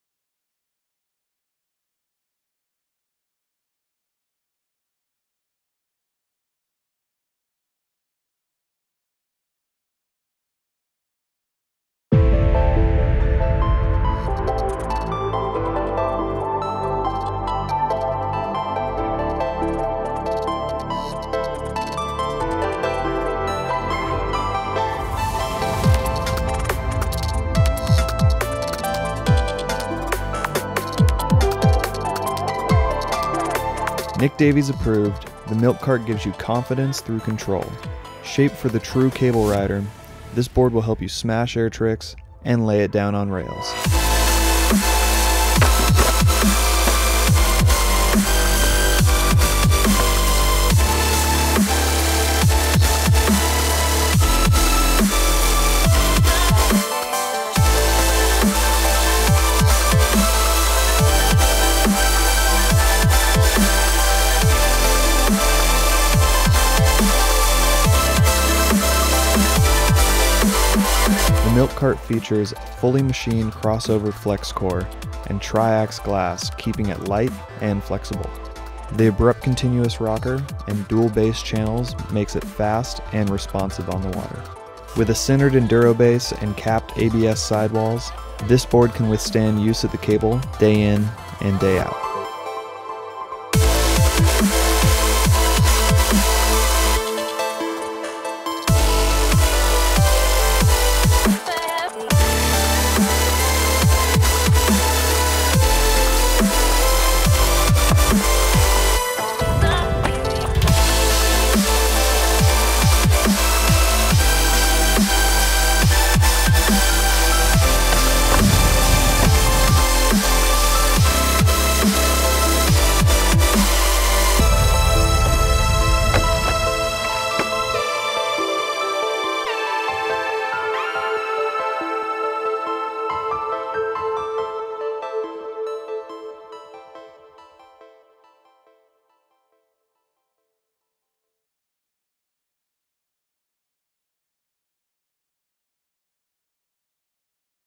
Hyperlite MilkCart - 2016 Nick Davies Signature Wakeboard
Confidence in control and shaped for the true cable rider, smashing air tricks and fun on features.

Mr. Davies and the design team at Hyperlite set out to create a new park shape that delivers wholesome goodness day after day. After a little vitamin D and some laps around the cable, the MilkCart was born. This new design takes all of the hard edging feel of Nick’s previous shapes and combines a new more flexible feel on rail. The MilkCart features a thinner, more durable Crossover Core without sacrificing full tip to tail flow channels and angled fins for maximum hold and response. Our first ever internal Cap Sidewall provides more durability than traditional ABS thanks to the additional layer of glass encasing the edge. The MilkCart is the perfect series for the hard charging cable rider looking for pop on air tricks as well as control and response on features.